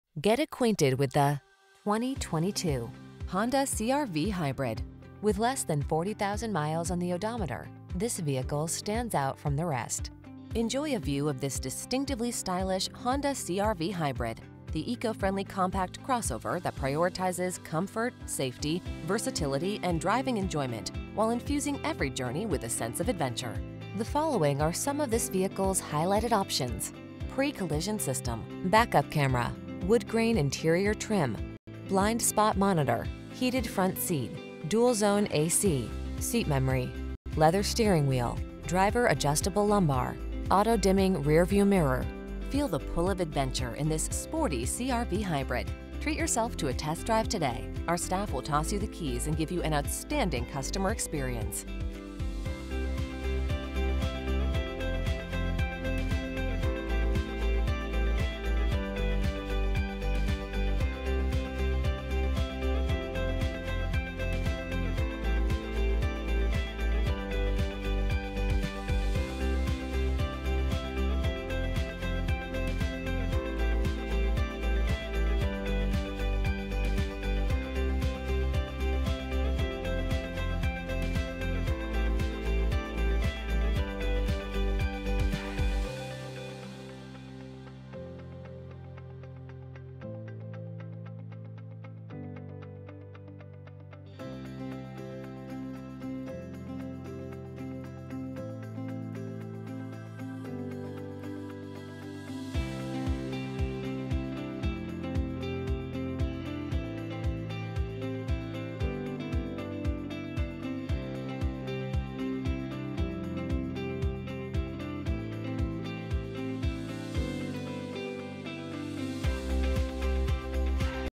2022 Honda CR-V_Hybrid TOURING AWD Henderson, Las Vegas, Laughlin, St George, Flagstaff NV
Pre-Owned 2022 Honda CR-V_Hybrid TOURING AWD available at Findlay Honda Henderson located in 933 Auto Show Dr, Henderson, Nevada, 89014. Servicing the Henderson, Las Vegas, Laughlin, St George, Flagstaff area.

Get acquainted with the  2022  Honda CR-V_Hybrid.  Enjoy a view of this distinctively stylish Honda CR-V Hybrid, the eco-friendly compact crossover that prioritizes comfort, safety, versatility, and driving enjoyment while infusing every journey with a sense of adventure.  The following are some of this vehicles highlighted options: Pre-Collision System, Back-Up Camera, Woodgrain Interior Trim, Blind Spot Monitor, Heated Front Seat(s), Dual Zone A/C, Seat Memory, Leather Steering Wheel, Driver Adjustable Lumbar, Auto-Dimming Rearview Mirror. Feel the pull of adventure in this sporty CR-V Hybrid. Treat yourself to a test drive today. Our staff will toss you the keys and give you an outstanding customer experience.